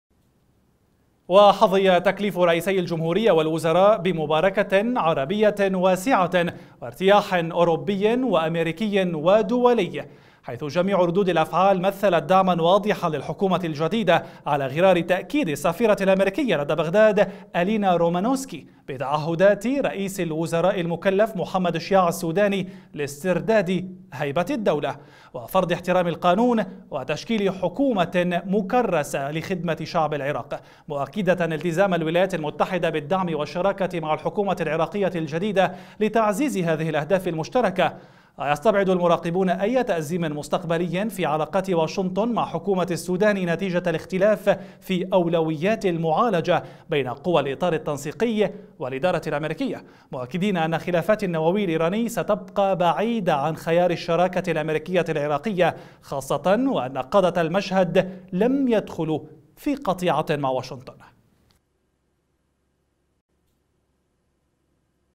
ترحيب عربي والتزام أمريكي لدعم التعهدات الأربعة للسوداني
وحظي تكليف رئيسي الجمهورية والوزراء بمباركة عربية واسعة وارتياح أوروبي وأمريكي و دولي، حيث جميع ردود الأفعال مثلت دعما واضحا للحكومة الجديدة، على غرار تأكيد السفيرة الامريكية لدى بغداد آلينا رومانوسكي، بتعهدات رئيس الوزراء المكلف محمد شياع السوداني لاستردادِ هيبةِ الدولة، وفرضِ احترامِ القانون وتشكيل حكومة مكرسة لخدمة شعب العراق، مؤكدة التزام الولايات المتحدة بالدعم والشراكة مع الحكومة العراقية الجديدة لتعزيز هذه الأهداف المشتركة، ويستبعد المراقبون أي تأزيم مستقبلي في علاقات واشنطن مع حكومة السوداني نتيجة الاختلاف في ألويات المعالجة بين قوى الاطار التنسيقي و الادارة الأمريكية، مؤكدين أن خلافات النووي الايراني ستبقى بعيدة عن خيار الشراكة الأمريكية العراقية، خاصة وأن قادة المشهد لم يدخلوا في قطيعة مع واشنطن.